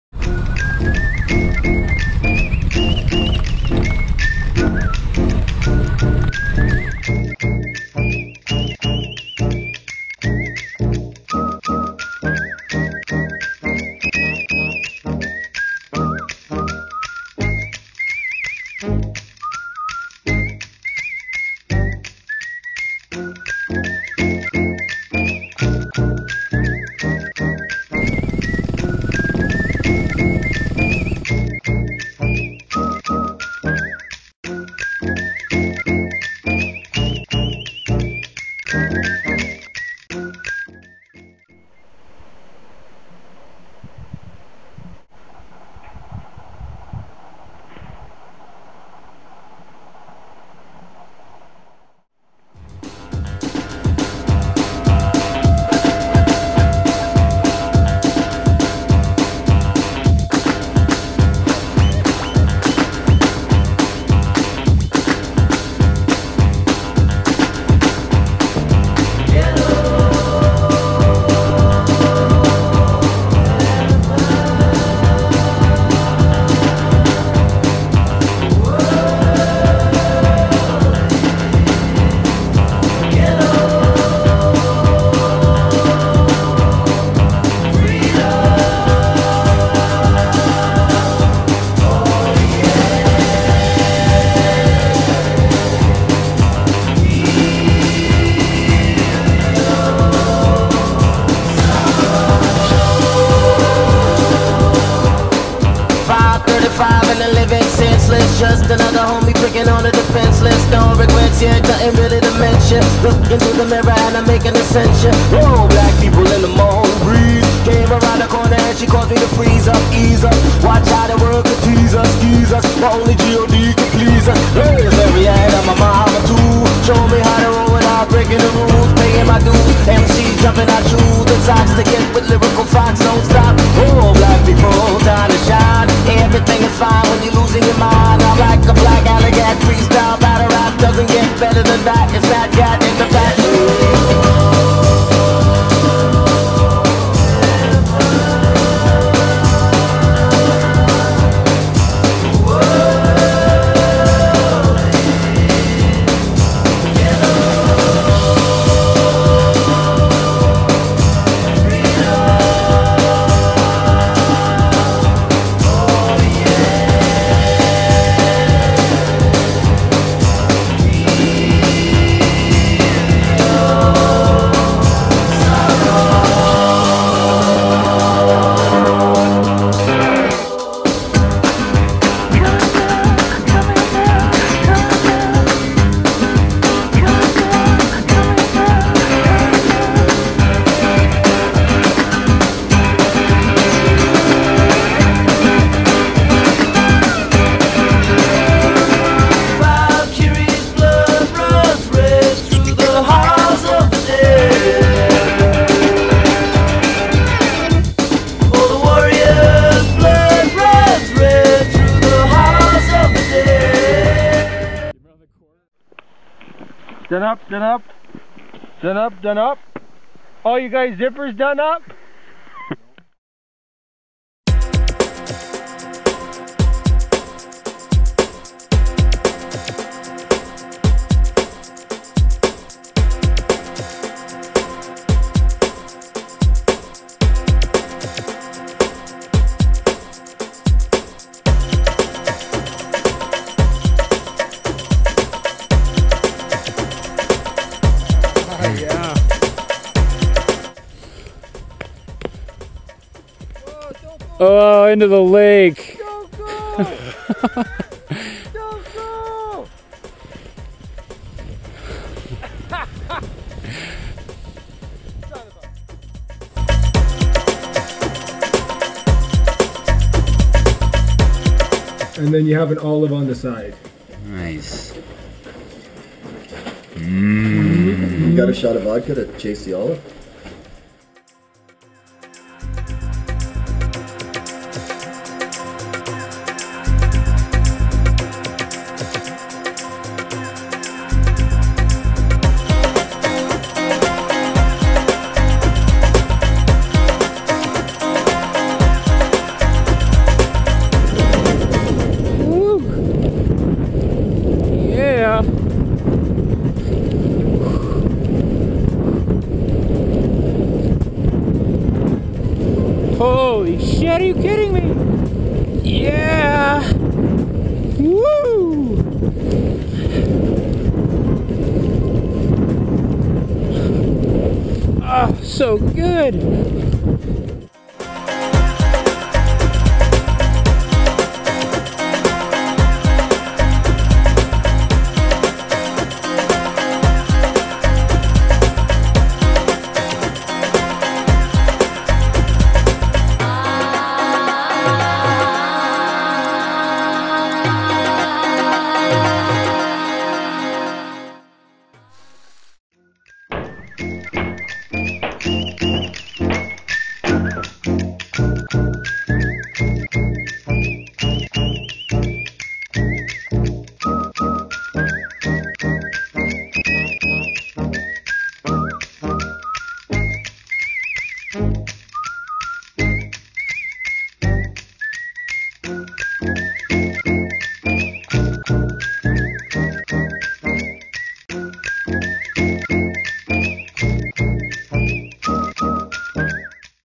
Selkirk Select
With friends in the Selkirk Range of British Columbia. Skiing, spirits and smiles. By Ryan Kuhn. Music by Nate Lott, K-os and Thievery Corporation.